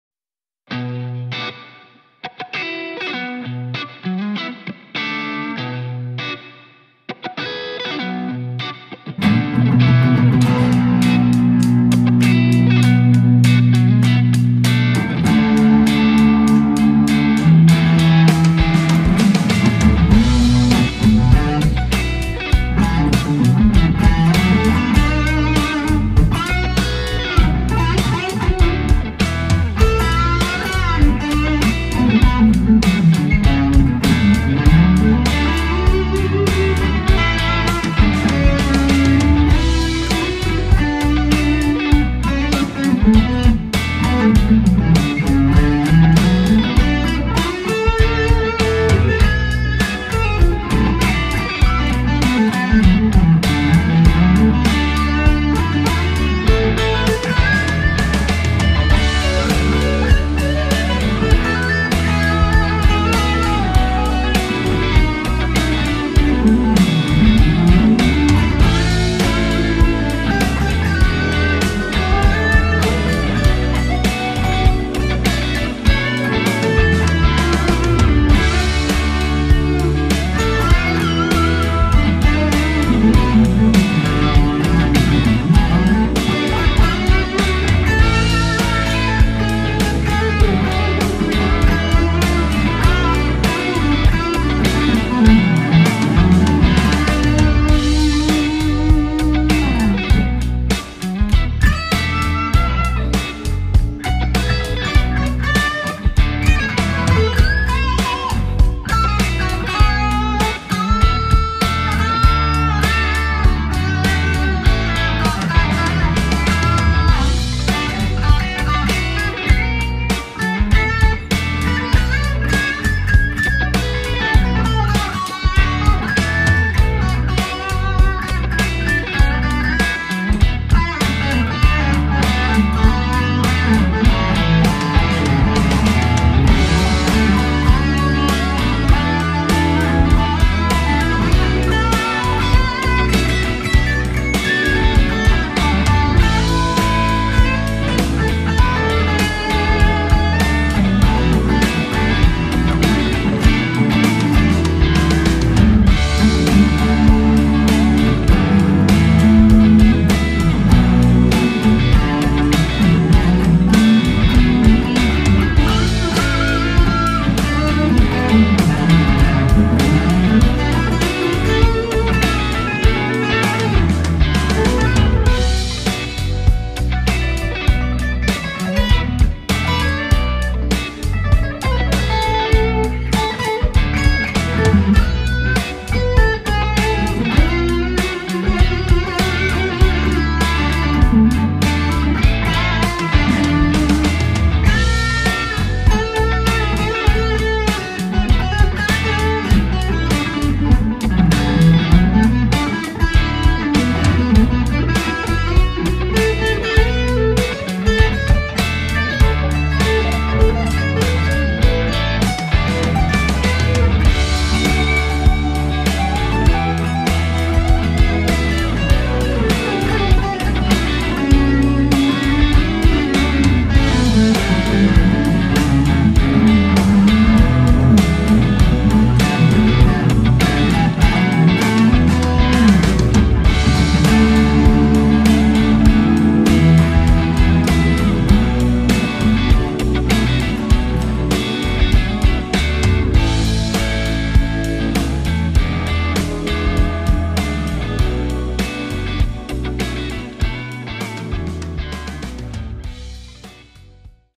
Soulful Blues Groove Jam
Guitar -- Gusta Tele HH
Amp -- Marshall MG10G
Video&Sound recorded -- iPhone 11 Pro (Enhanced sound in GarageBand App)